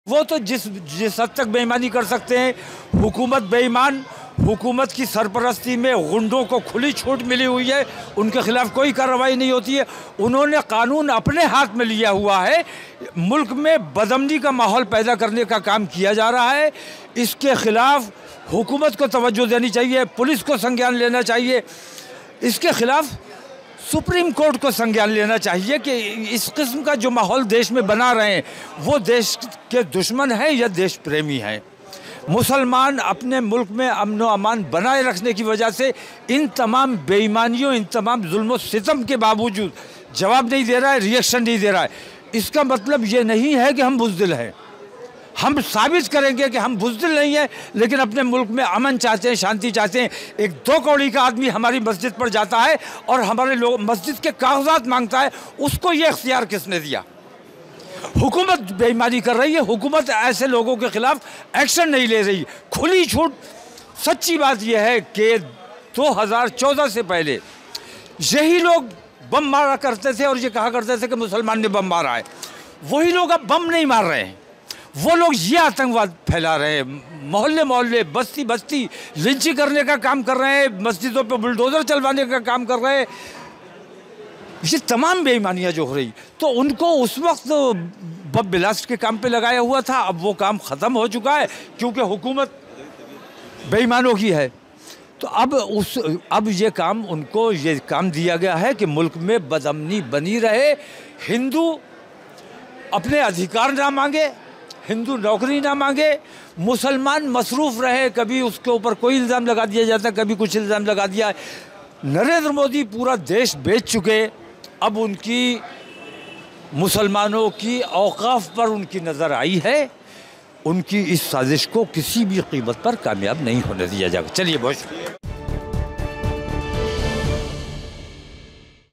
Navratri में मीट की दुकान बंद होने के सवाल पर Maulana Tauqeer Raza ने दी प्रतिक्रिया
नवरात्रि में मीट की दुकान बंद होने के सवाल को लेकर मौलाना तौकीर रज़ा ने कहा कि यह स्पष्ट है कि सरकार की छत्रछाया में गुंडों को खुली छूट मिली हुई है। वे कानून अपने हाथ में लेकर देश में अशांति फैला रहे हैं, लेकिन उनके खिलाफ कोई कार्रवाई नहीं होती। सरकार और पुलिस को इस पर ध्यान देना चाहिए, और सुप्रीम कोर्ट को भी इसका संज्ञान लेना चाहिए। मुसलमान देश में शांति बनाए रखने के लिए धैर्य दिखा रहे हैं, लेकिन इसका मतलब यह नहीं कि हम कमजोर हैं। हम अमन चाहते हैं, लेकिन अन्याय बर्दाश्त नहीं करेंगे।